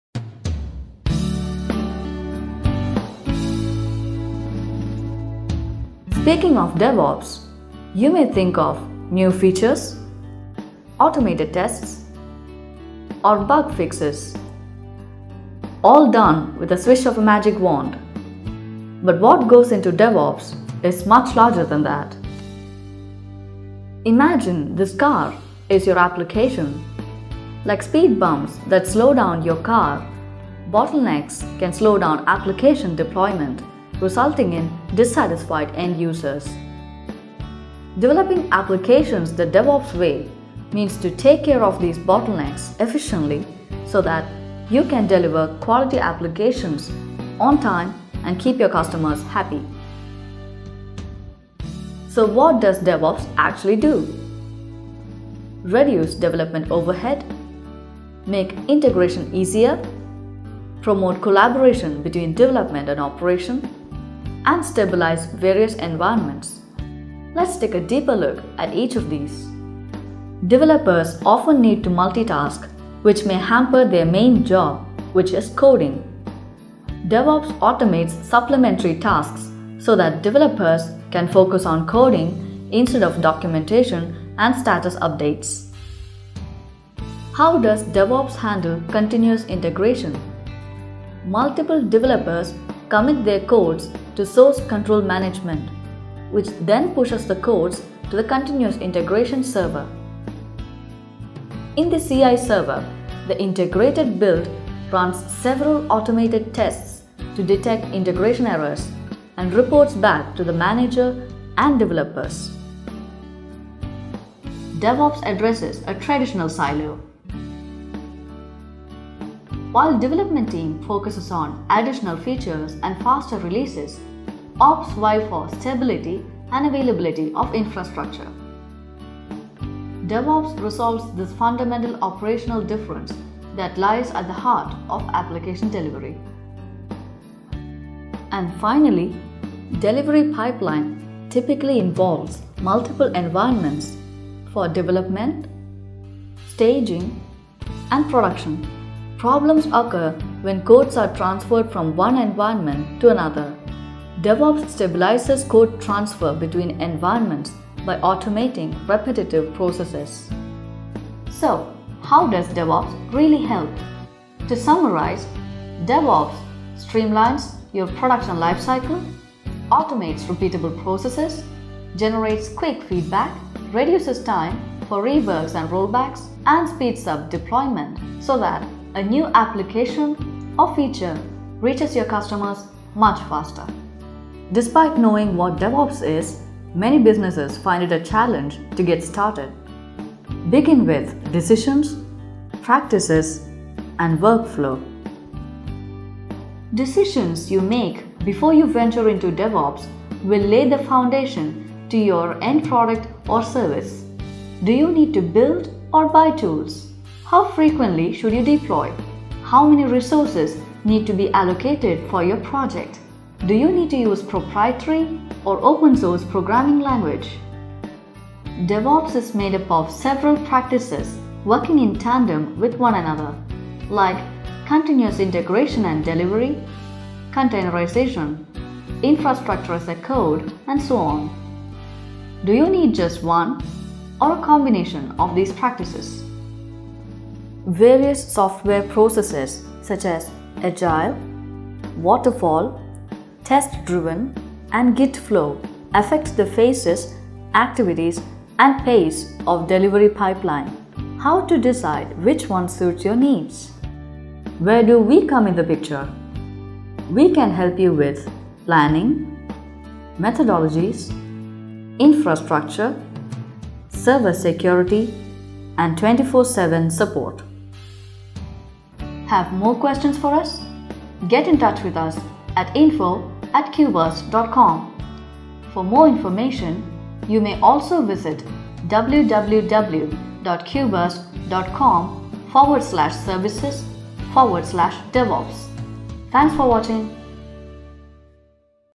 Product Development: The DevOps Way
Developers often need to multitask, which hampers their main job which is coding. DevOps automates supplementary tasks so that developers can focus on coding instead of documentation and status updates. Learn the various benefits of implementing a DevOps strategy.